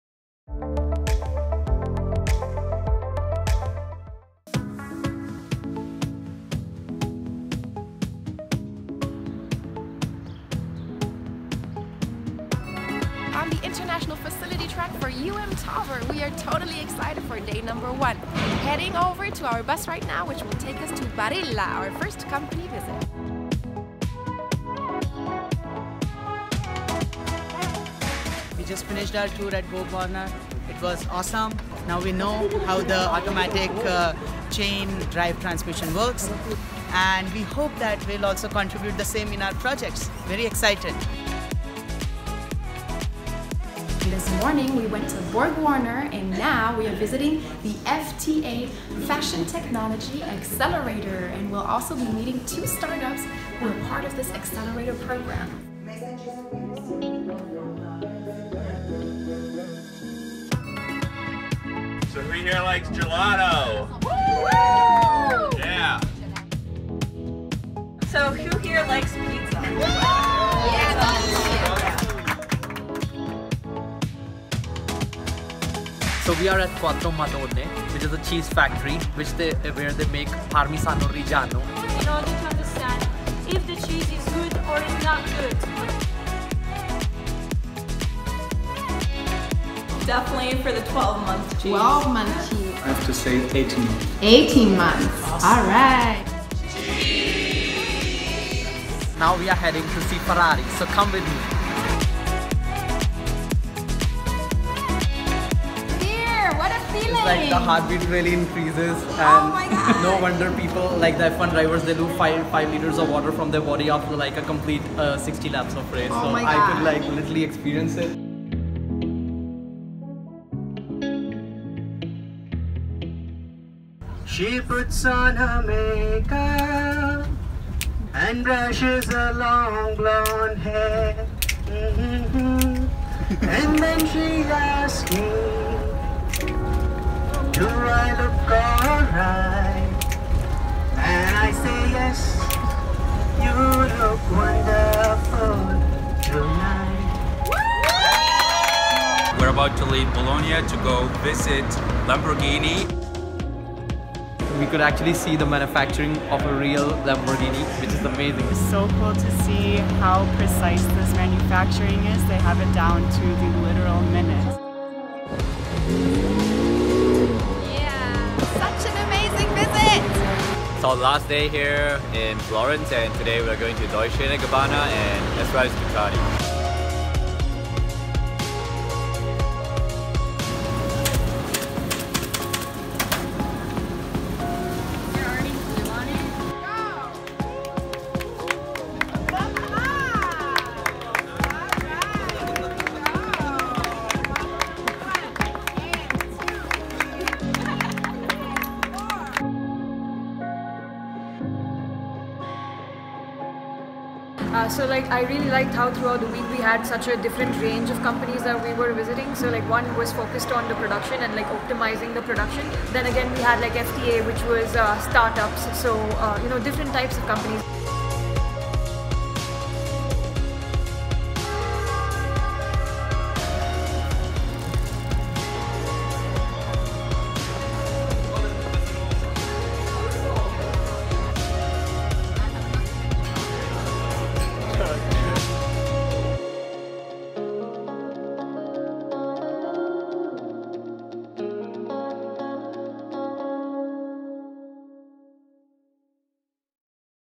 Tauber 2019 International Trek to Italy
On this International Facility Trek to Italy, including the cities of Milan, Bologna, and Florence, students went "to the Gemba" of the most prominent Italian companies in the industries of engineering, fashion, food and transportation.

Besides visiting Borg Warner, Gucci, Ducati, and a Milan-based start-up, some highlights included a look behind the scenes of the pasta making process at the Barilla factory, a visit to the Lamborghini museum and manufacturing plant as well as a tour during which the students admired its rivaling counterparts inside the panoramic Ferrari factory and outside on the Fiorano racetrack.

In addition, students enjoyed two pre-selected optional cultural activities in Milan and discovered the beauty of each of the three cities during plenty of free time in the late afternoons and evenings.

Program at a glance:

Tours: 9 company visits and 1 start-up

Plant tours included a behind-the-scenes look at each facility's continuous improvement initiatives, use of technology, and workforce best practices. Gained improvement ideas for students to use in their toolbox.

Cultural activities: La Scala theater and museum tour the ʻLast Supperʼ, Sforza castle, and the Duomo

Length of program: 1 week

Number of participants: 20 students, 2 faculty members